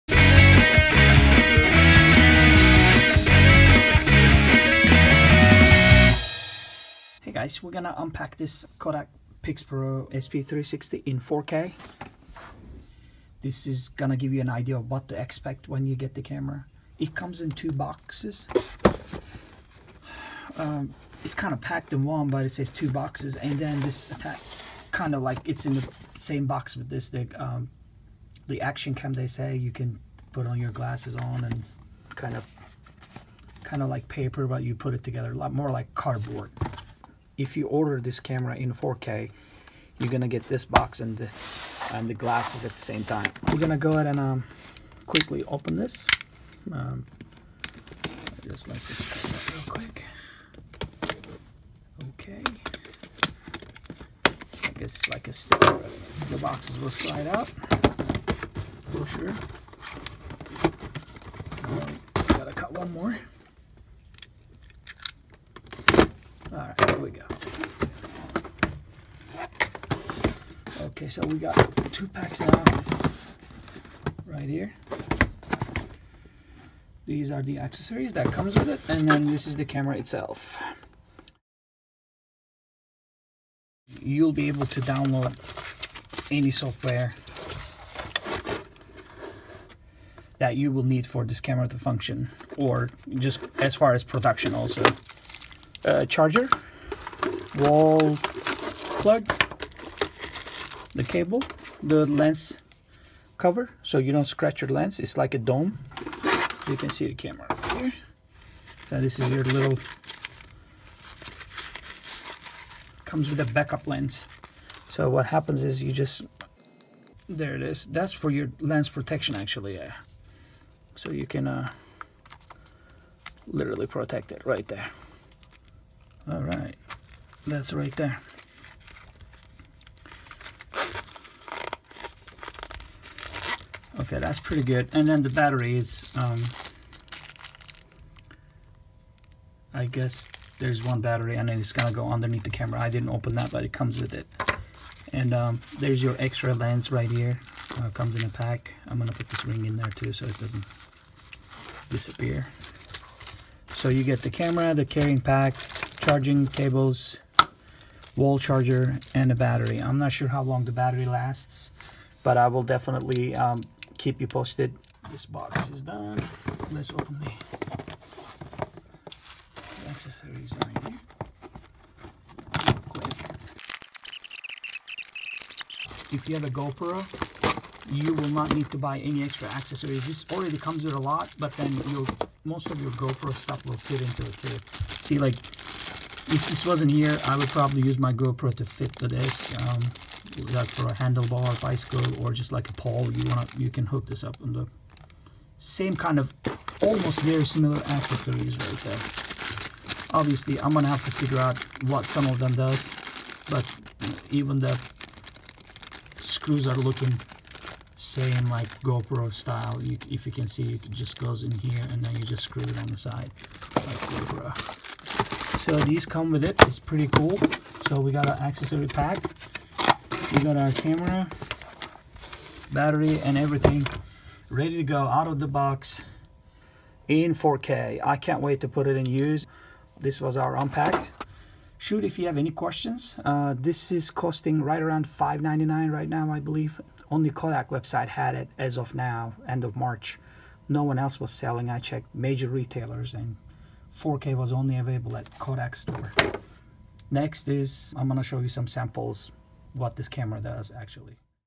Kodak Pixpro SP360 4K Camera | Unboxing | Premier Pack
This video will give you a good idea of what you will get with 4K premier pack. Sample videos recorded with this camera are below as well as a very detailed video of the camera. Camera comes with a free software download from Kodak and I have already posted links below to videos of how to import, edit, unfold and export videos with the two Kodak software.

As of April 12, 2016 Kodak is the only one selling the 4K version of the camera.

The premier pack ($499) includes:
Standard Housing
Curved / Flat Adhesive Mount
Bar Mount
Suction Cup Mount
L-Type Bracket Mount
L-Type Bracket Adhesive Mount
Square Double-Sided Adhesive
Accessories Tool
Lens Ring
Lens Cover
Protective Cover
Battery Charger
USB Cable
AC Adapter
Battery
Carrying Case
Small Lens Cover Case
Cleaning Cloth